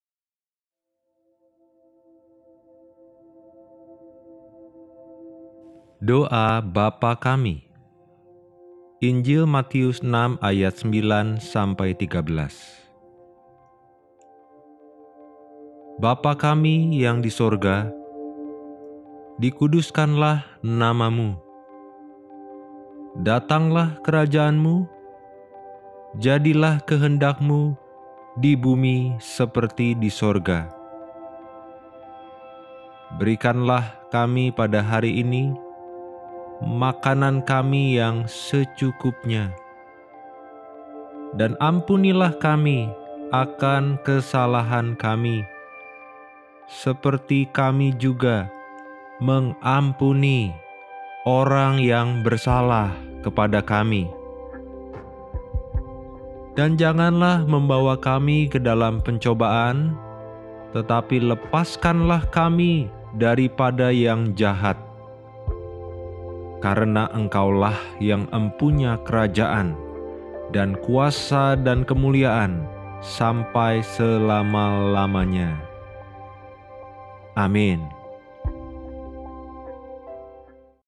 Doa Bapa Kami (Matius 6:9-13) Audio Doa Alkitab
DOA BAPA KAMI

Matius 6:9-13
6:9 Karena itu berdoalah demikian: Bapa kami yang di sorga, Dikuduskanlah nama-Mu,
6:10 datanglah Kerajaan-Mu, jadilah kehendak-Mu di bumi seperti di sorga.
6:11 Berikanlah kami pada hari ini makanan kami yang secukupnya
6:12 dan ampunilah kami akan kesalahan kami, seperti kami juga mengampuni orang yang bersalah kepada kami;
6:13 dan janganlah membawa kami ke dalam pencobaan, tetapi lepaskanlah kami dari pada yang jahat. [Karena Engkaulah yang empunya Kerajaan dan kuasa dan kemuliaan sampai selama-lamanya. Amin.]